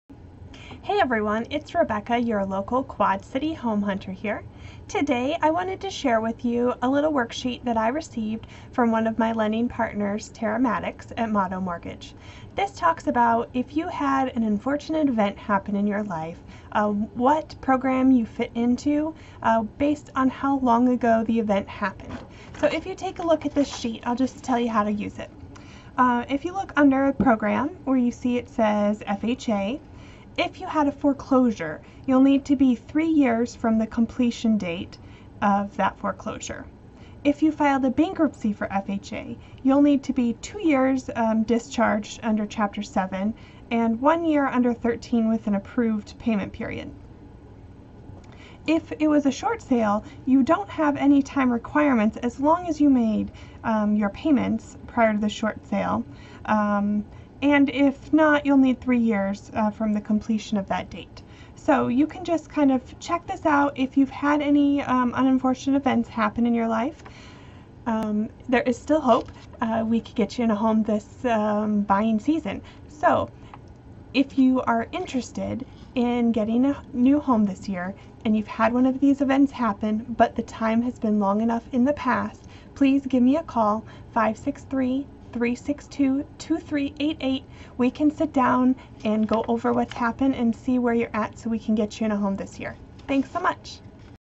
Unfortunate Credit Events
Time from unfortunate event to purchasing a home again